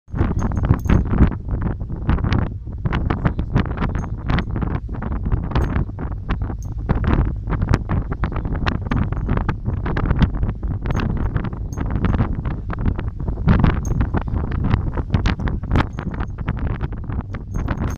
Marsa the Dog and her flying ears on board
Στο πρώτο ταξίδι της Μάρσας με καράβι τα αυτιά της χορεύουν σαν τρελά στον αέρα!